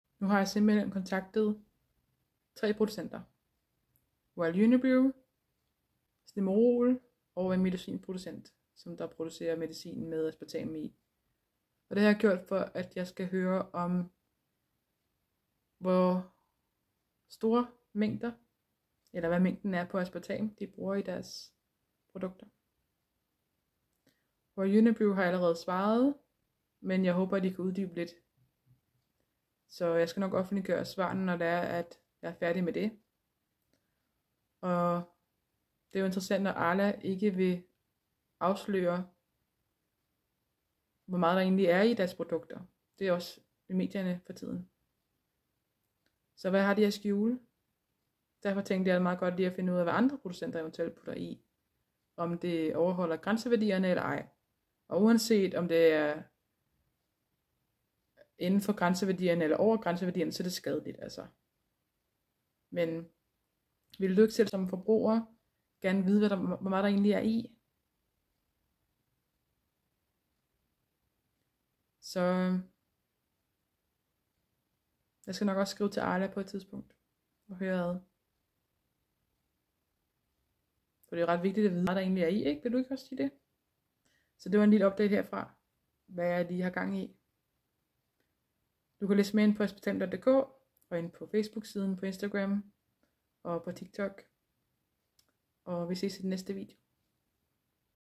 Royal Unibrew, Stimorol og medicinproducent er kontaktet vedr. spørgsmål om Aspartam mængder
Lad os se om de giver mig svar på mine spørgsmål om hvor meget Aspartam der er tilføjet i deres produkter hvor der er Aspartam i.

Se også min video om Royal Unibrew svar om deres brug af Aspartam her